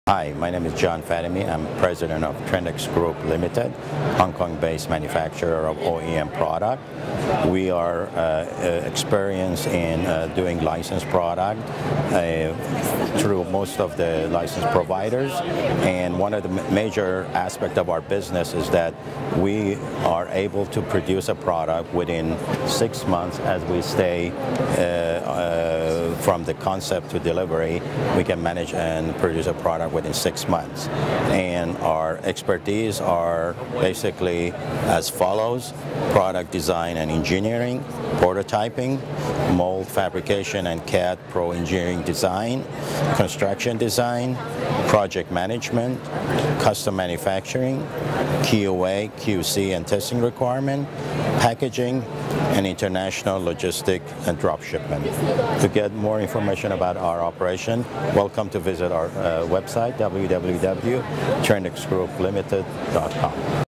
TRENDEX
TRENDEX takes all the headaches out of inventing a new toy or game. You come up with the ideas, Trendex takes care of the manufacturing for you, handling everything including testing.  As seen at Toy Fair, February 2014. Videography by Robert Poswall. Editing by Alison Marek. See more new toys and videos at ToyDirectory.com/TDmonthly.com